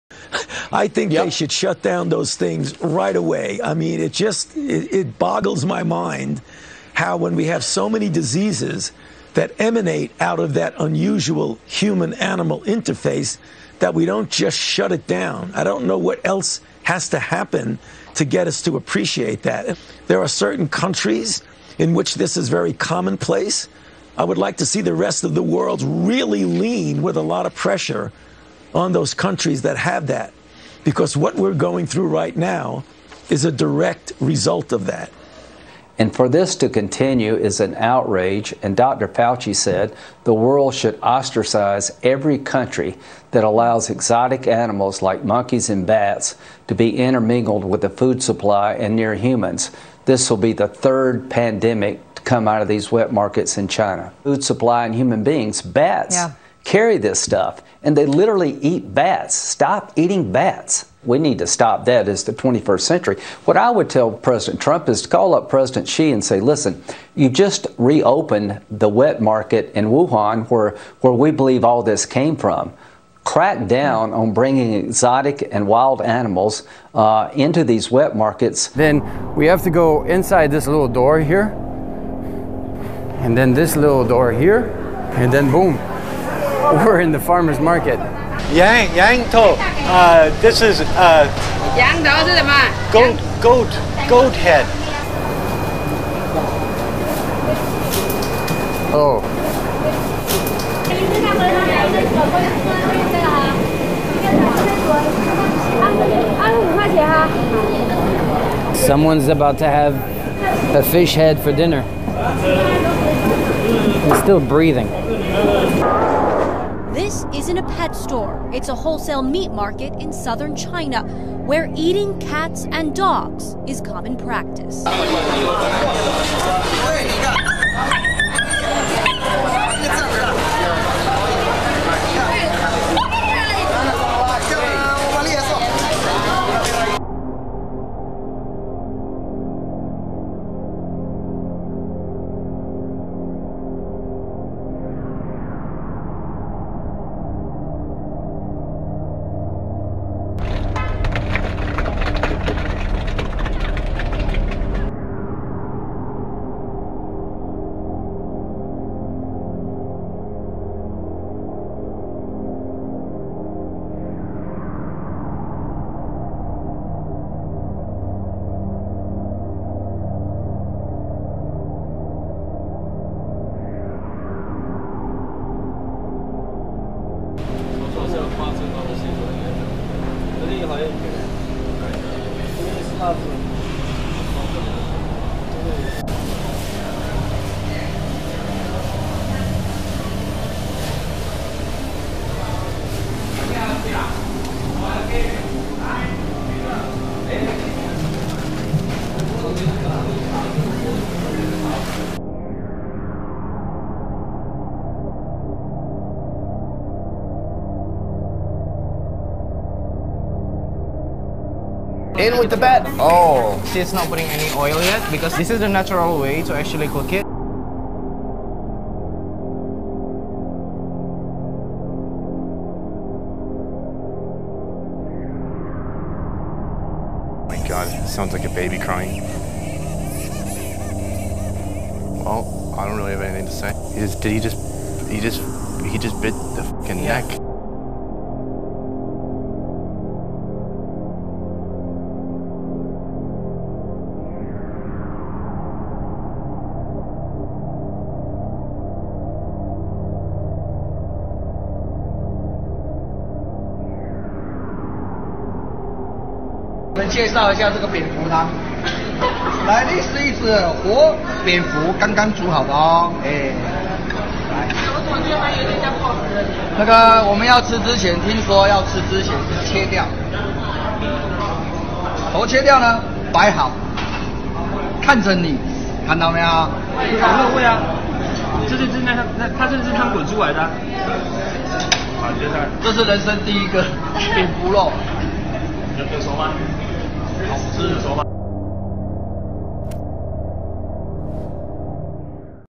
Dr. Fauci And Lindsey Graham Blast Chinese Wet Markets
During separate interviews on Fox News Dr. Fauci and Lindsey Graham voiced their criticism of China reopening wet markets across the country.